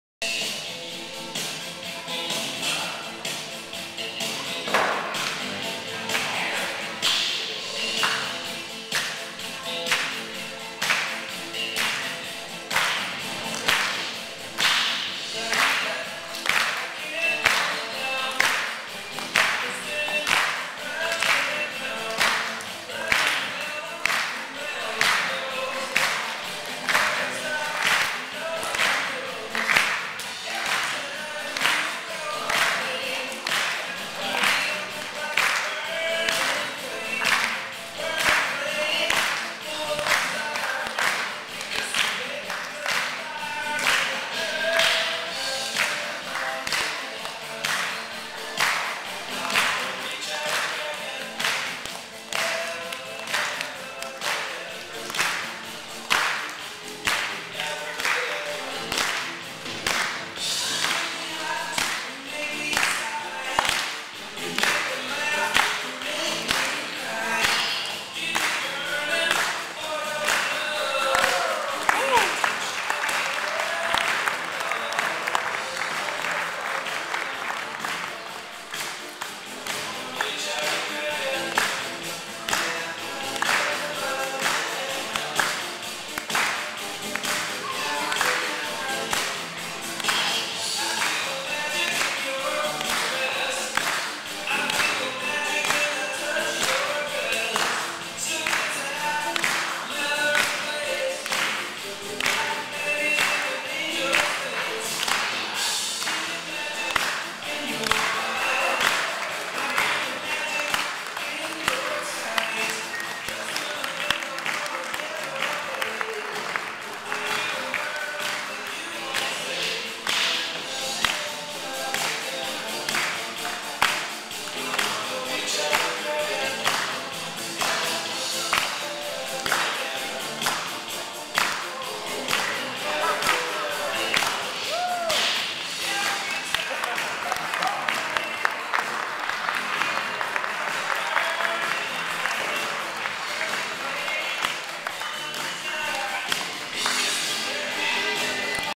Abracadabra, Peirce Elementary School Circus, 2010
For the last three years, the kids of Peirce Elementary School have put on a fabulous circus.  This is the magic show from the evening performance.